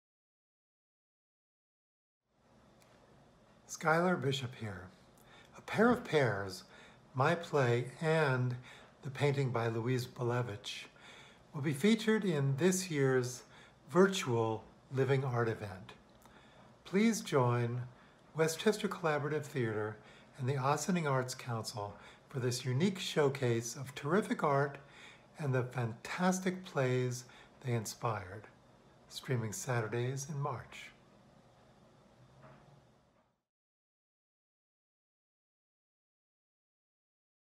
Schuyler Bishop - A Pair of Pears
Playwright Schuyler Bishop discusses his play A Pair of Pears, inspired by the painting of the same name by Louise Belevich.